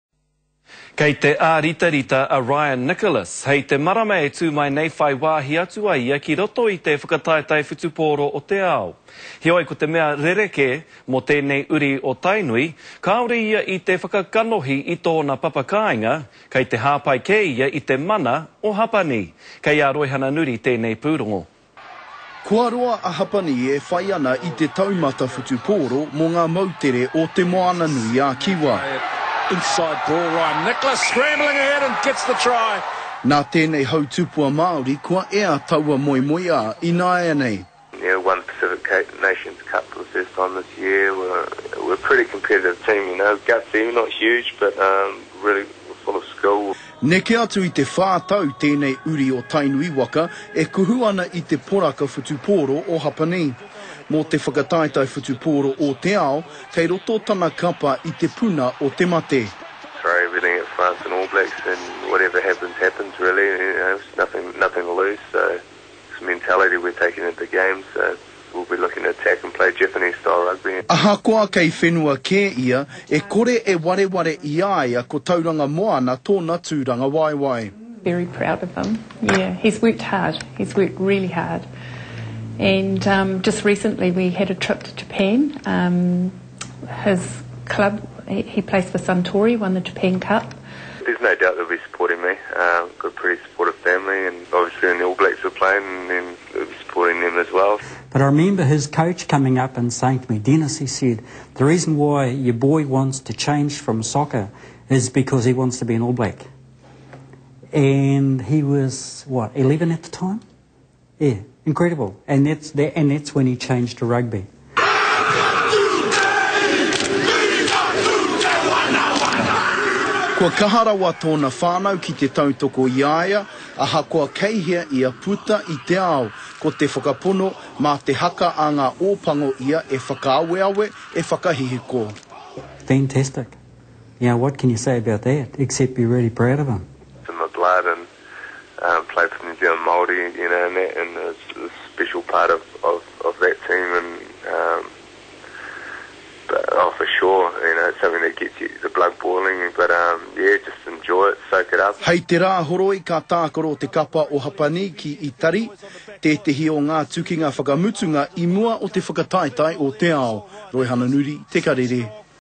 Maori plays for Japan in RWC
Kei te aaritarita a Ryan Nicholas - hei te marama e tu mai nei whai wahi atu ai ia ki roto i Te Whakataetae Whutuporo o te Ao. Heoi ko te mea rereke mo tenei uri - kaore ia i te whaka-kanohi i tona papa kainga - kei te hapai ke ia i te mana o Hapani.